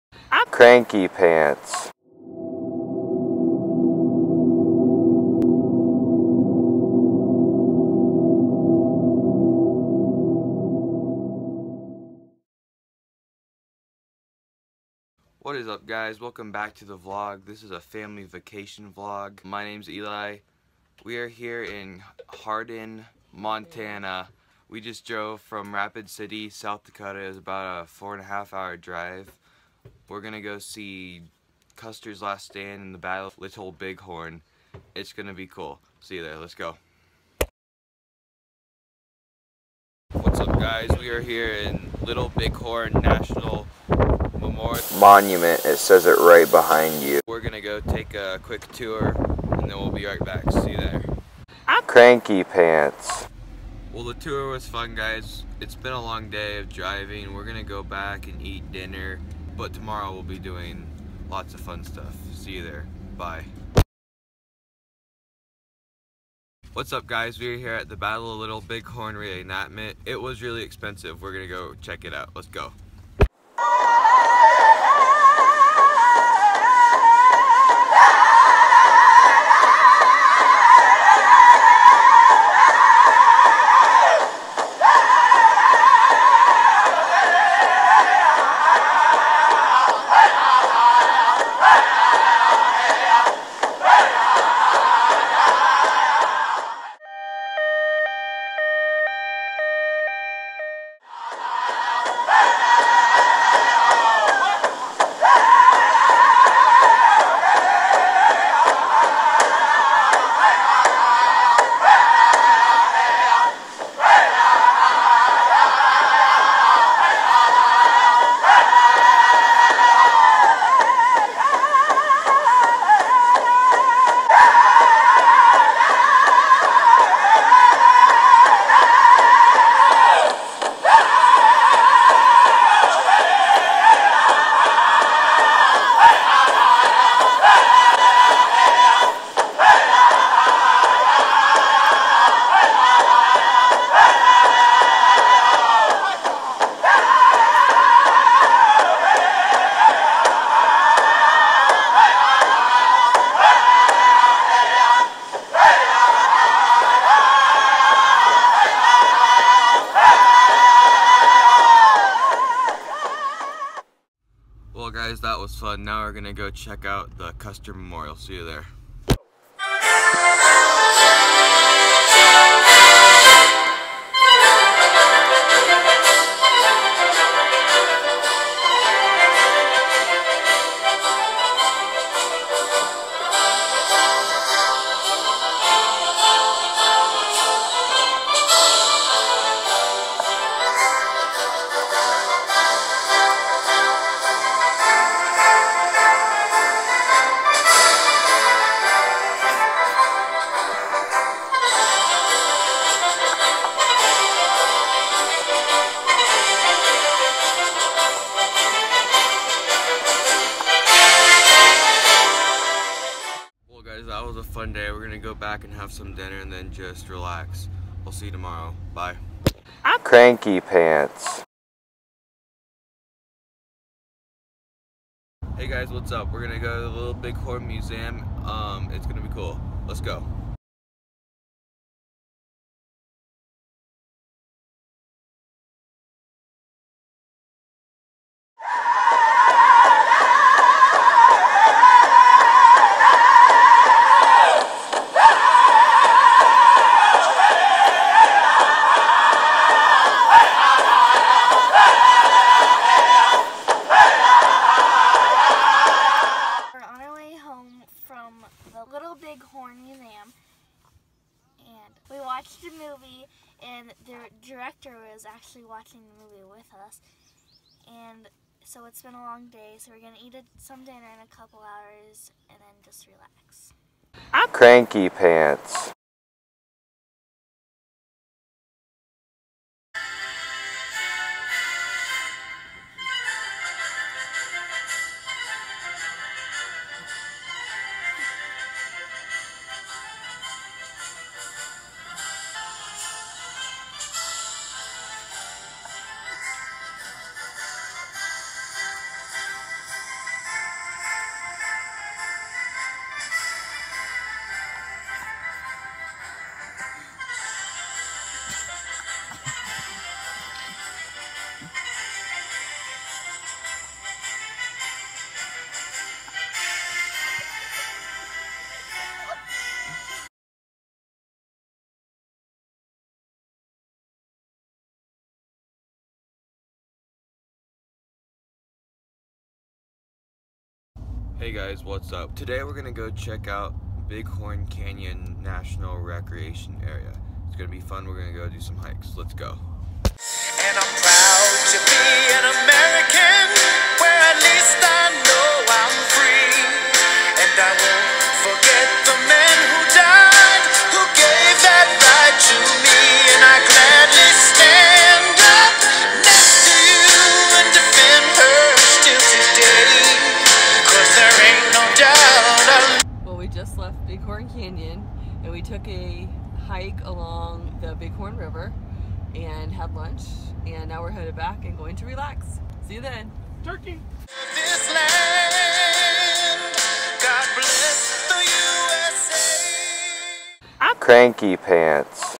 CUSTER, CHEYENNE, AND CRANKY PANTS
We go to The Battle of Little Bighorn and Bighorn Canyon. One night we meet a professional dancer, Yollie. She has decided to stay and dance for us for the rest of the trip.